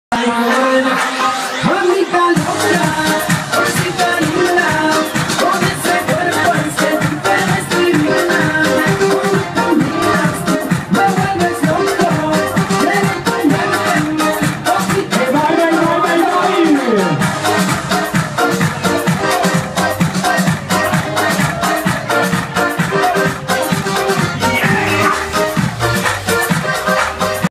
New Carrubo voice Vito Lolaico
Sabato 8 giugno the voice Vito Lolaico presso New Carrubo Agropoli